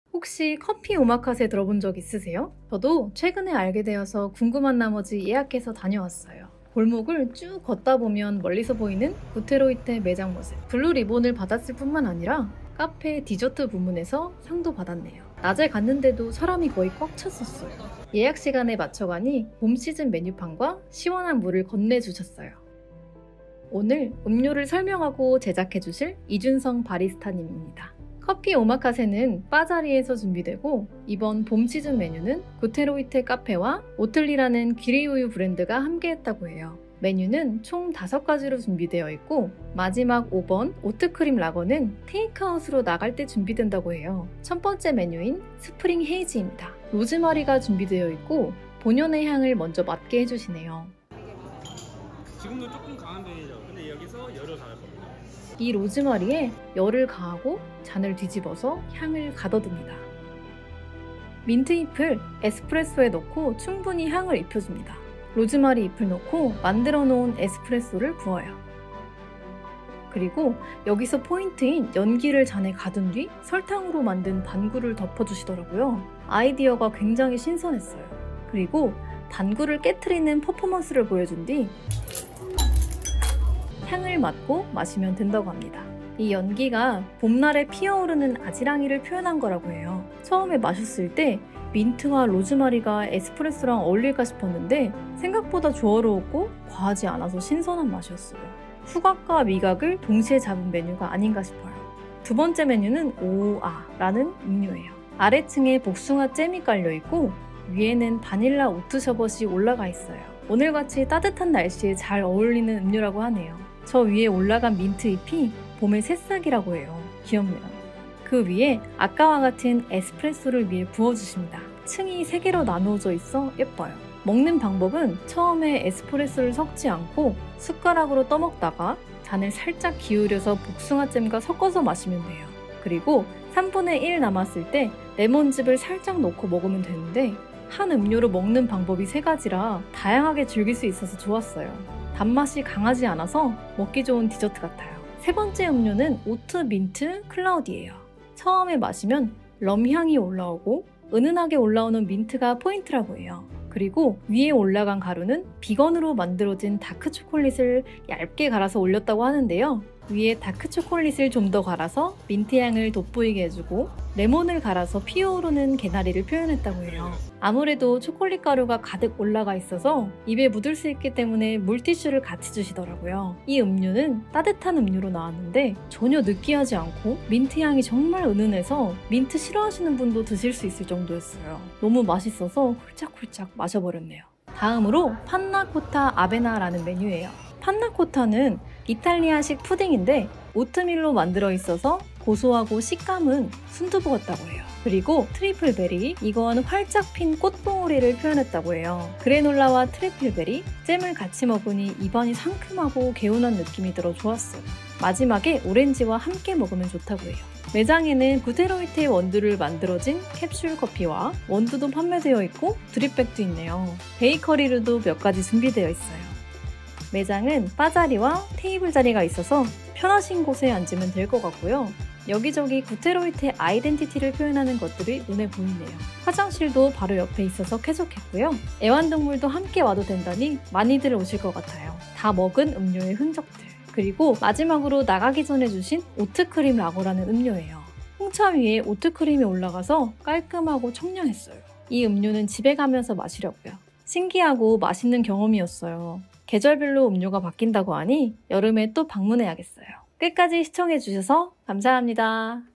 아니 이런곳이 있어?? 봄 메뉴 맛보고 여름도 필수로 가야겠다고 생각한 곳! 카페오마카세 | 구테로이테
일상과 리뷰를 공유하는 '리뷰에뜨'입니다

정말 신기하고 재밌고 맛있는 경험을 하고 와서 여러분께 공유드려요

- 보다 알고싶은 제품이 있다면 댓글로 남겨주세요!❤️